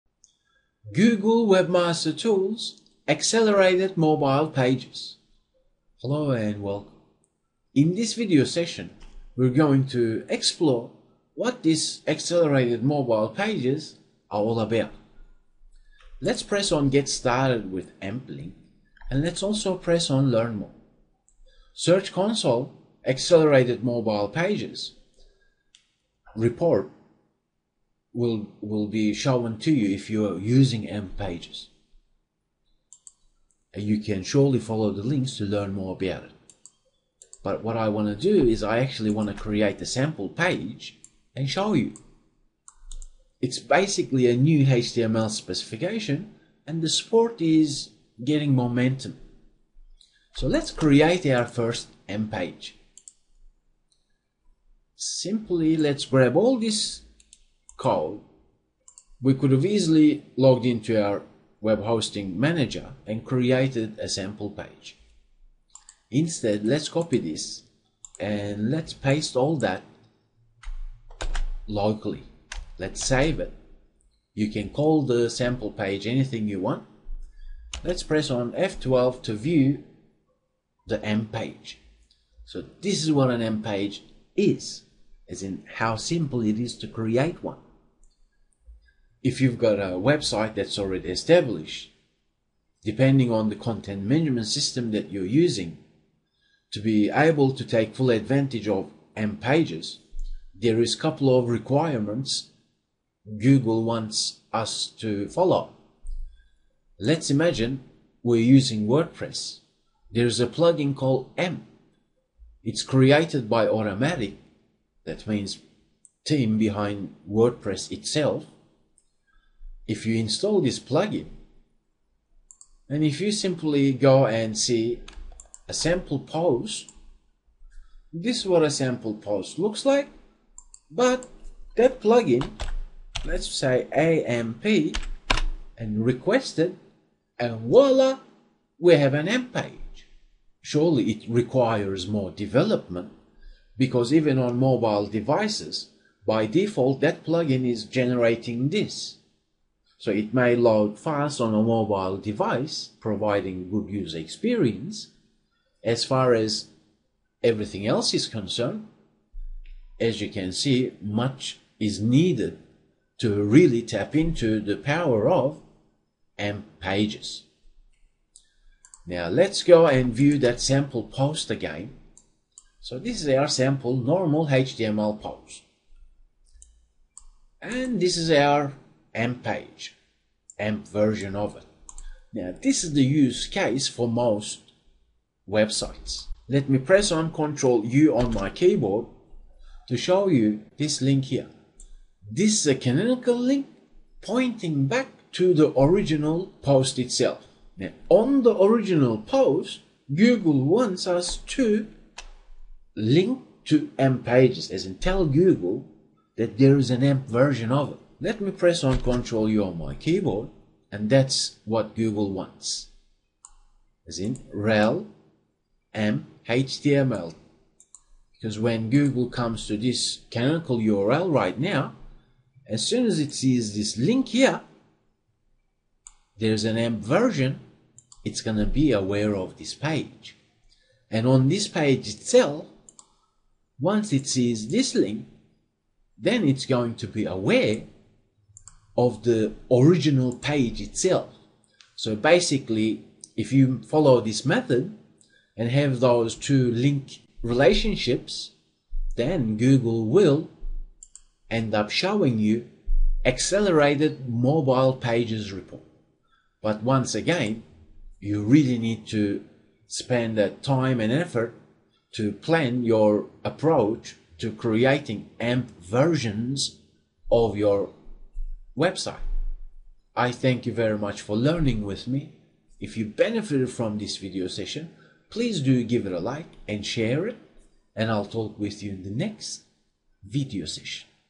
Google Search Console Accelerated Mobile Pages AMP
Google Search Console Accelerated Mobile Pages (AMP) video lesson explores AMP pages option in Search Console, basics of AMP creation.

Accelerated mobile pages (AMP) is a standard for creating fast-loading pages that look good on mobile devices.

Accelerated Mobile Pages (AMP) is an open source standard that allows you to create web pages that load quickly on mobile browsers. To help provide a better mobile user experience, you can create AMP versions of your landing pages.